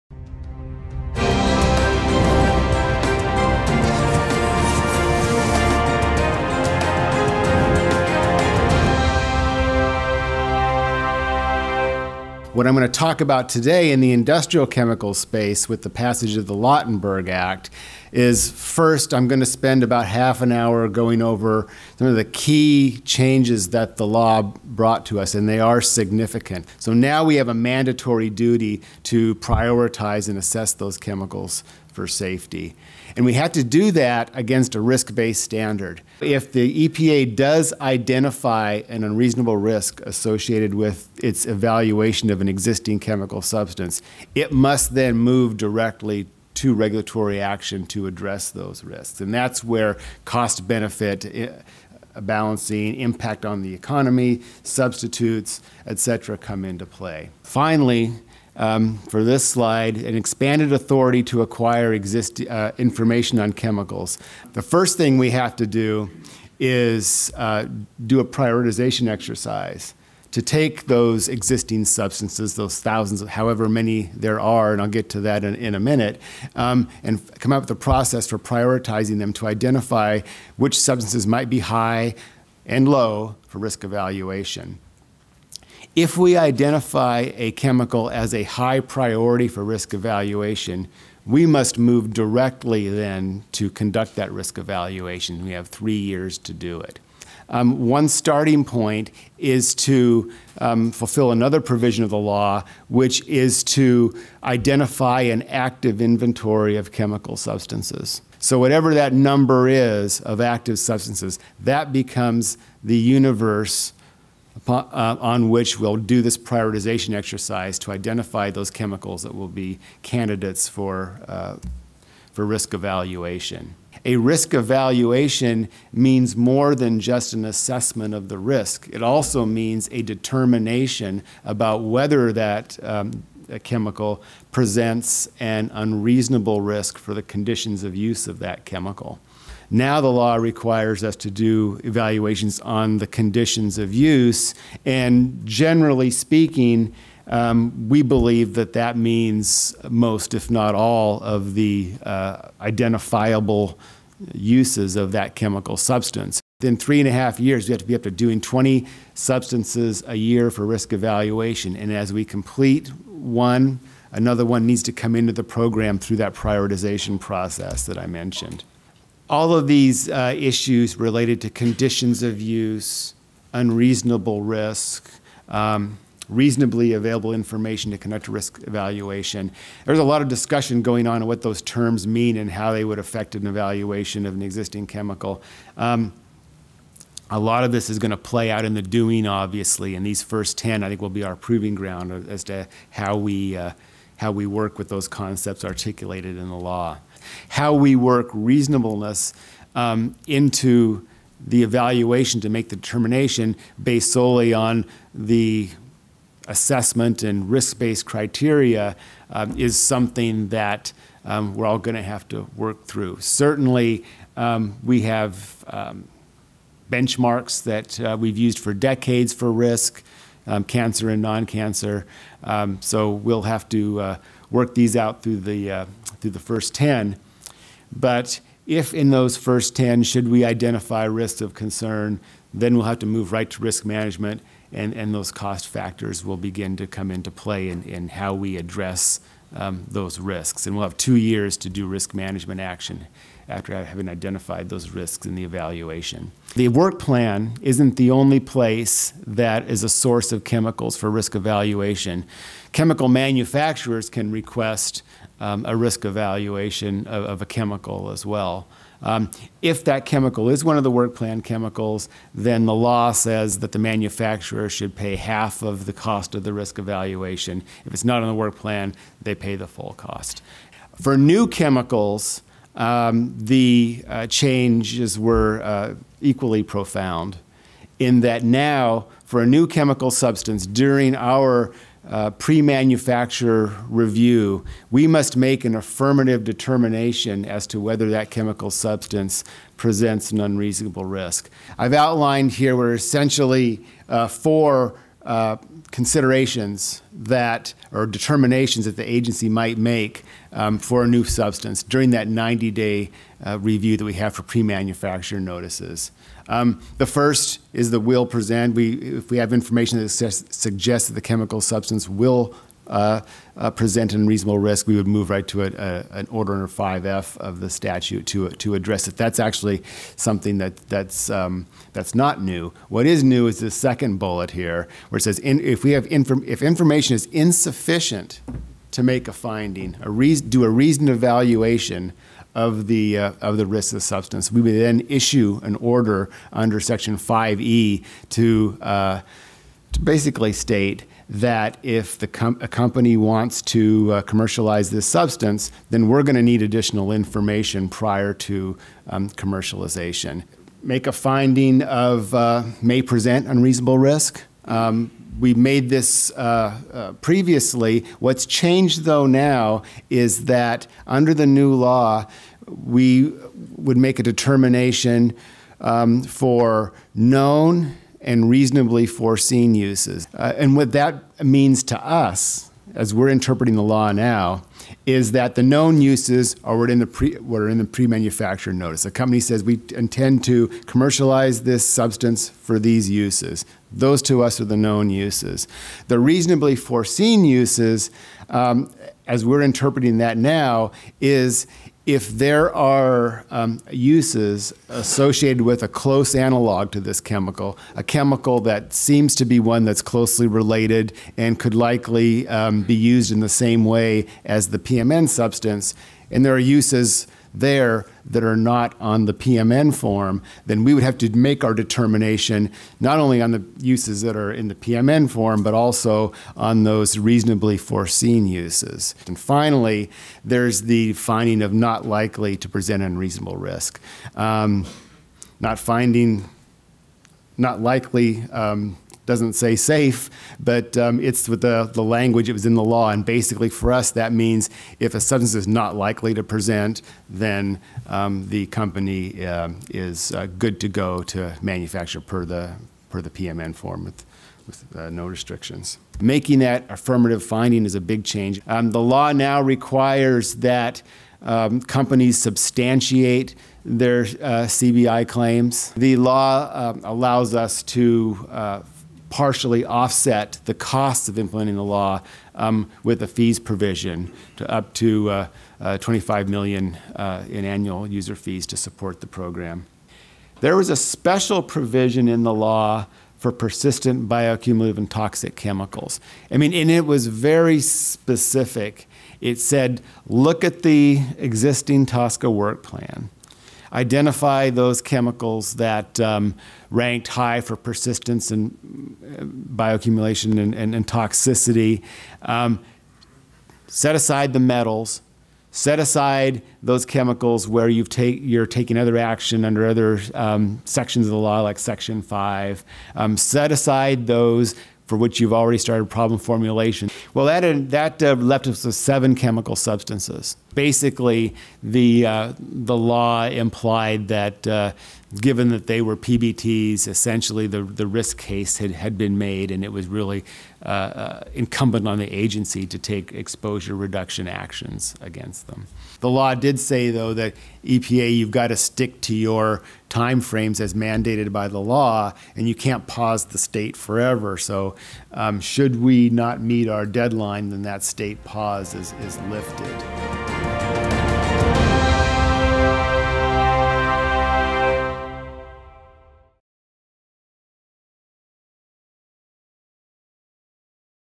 The Frank R. Lautenberg Chemical Safety for the 21st Century Act
Jeff Morris of the US EPA provides an overview of The Frank R. Lautenberg Chemical Safety for the 21st Century Act - the amendment of the Toxic Substances Control Act (TSCA). Among others Jeff Morris addresses the prioritization of chemicals for risk evaluation, the risk-based safety standard, TSCA work plan, manufacturer requested assessments, costs, new chemicals – affirmative finding and persistent, bioaccumulative and toxic chemicals